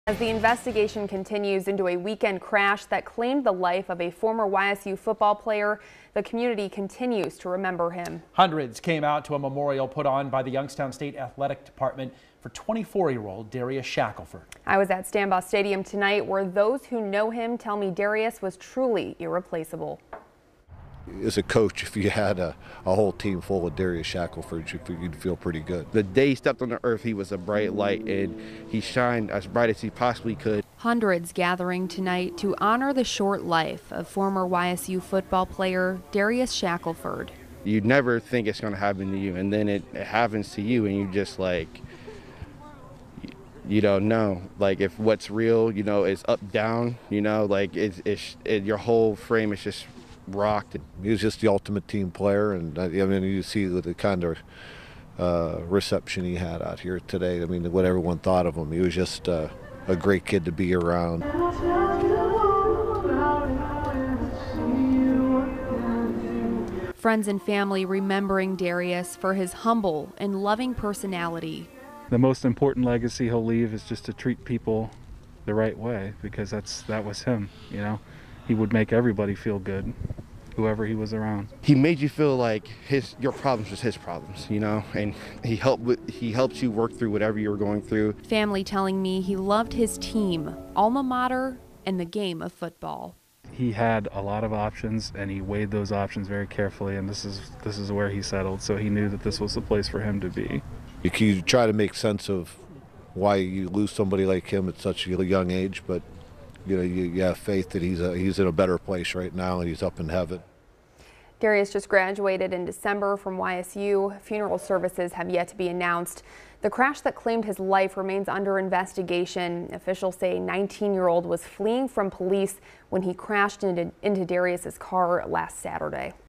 Hundreds turn out at vigil for former YSU football player killed in car crash
Hundreds of people came out to a memorial put on by the Youngstown State Athletic Department for former YSU Football Player 24-year-old Darius Shackleford, who was killed in a car crash late Saturday night.
Erin Simonek spoke with his family and friends about what he meant to them.